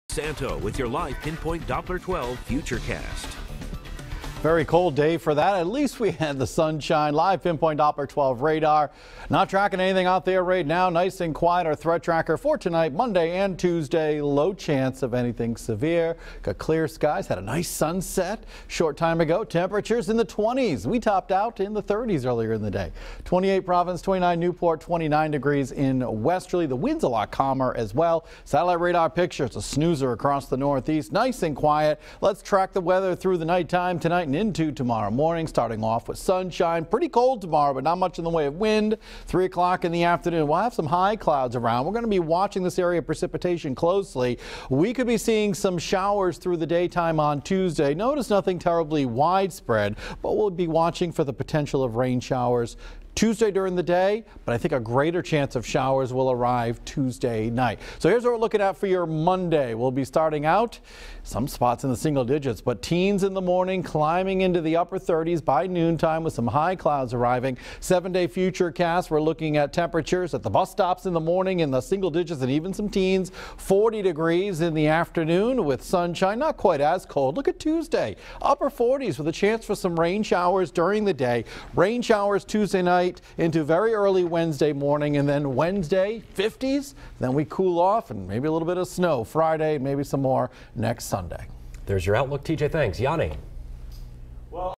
Live Pinpoint Doppler 12 7Day Futurecast
T.J. Del Santo's Live Pinpoint Doppler 12 7Day Futurecast from 3/5/17.  Bit of a warm up ahead.